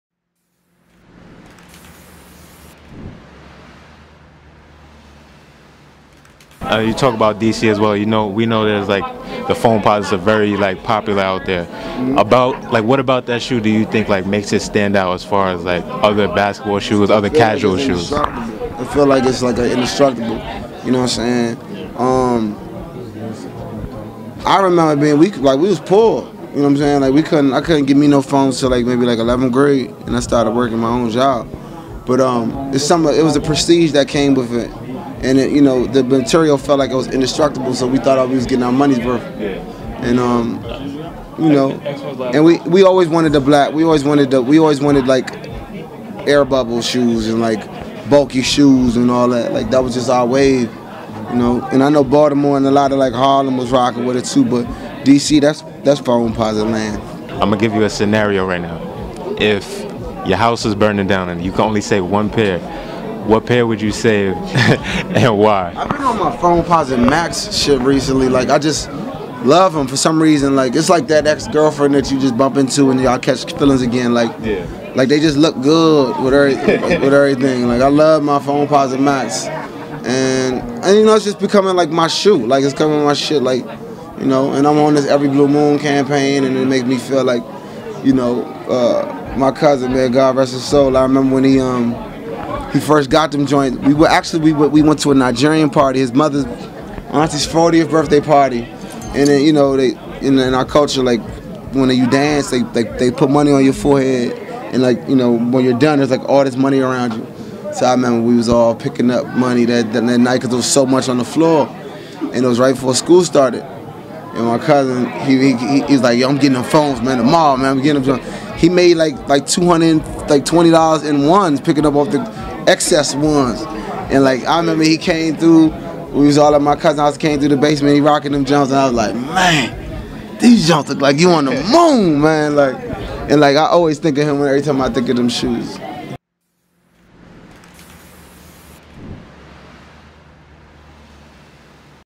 Wale: "D.C. Is Foamposite Land"
- Known for his crafty wordplay and artistic mind, Wale was able to sit and chat with us to discuss some of his favorite shoe models. Hailing from D.C., it's to no one's surprise that Wale would be interested in the classic Foamposite model from Nike.

Mentioning other areas that wear the Foamposite as a staple of sneaker culture such as Baltimore and Harlem, Wale makes sure to let fans know that D.C. is the land of the Foamposite shoe.

Reminiscing about his childhood and how difficult it was to get foams at an early age, he also refers to the shoe being somewhat indestructible.